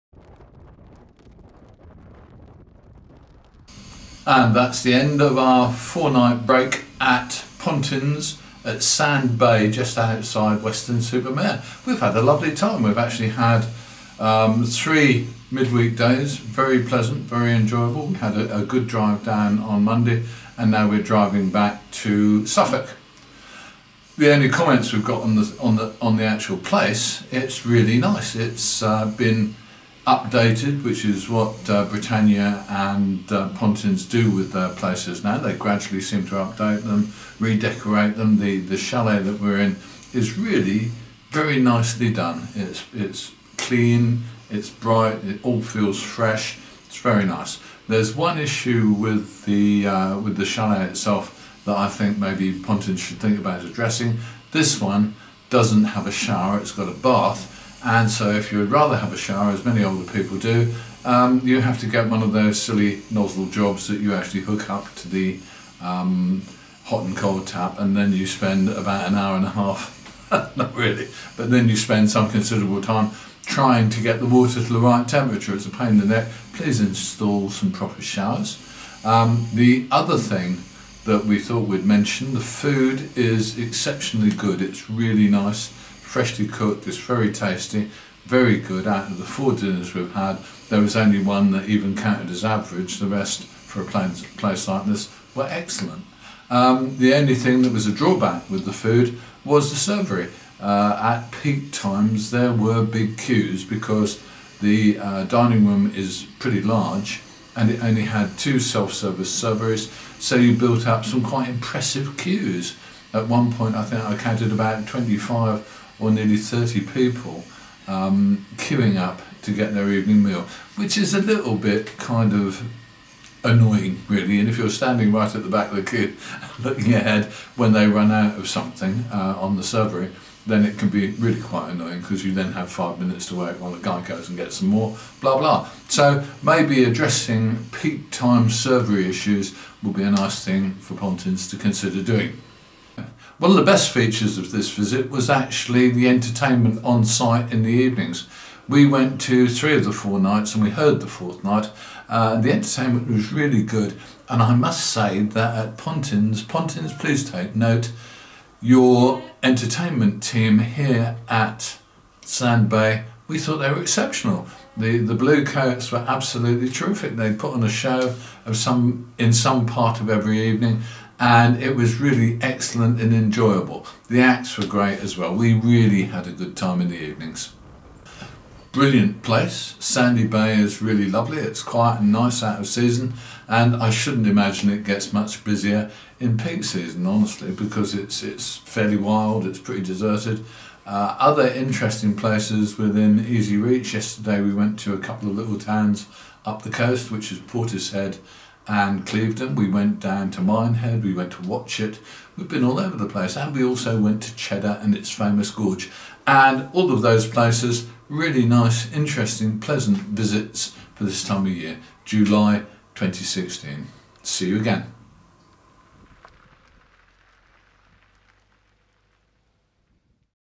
Pontins Sand Bay short break - summary, pt 3
A summary of our 4-night short break at Pontins Sand Bay holiday park, just outside Weston-Super-Mare.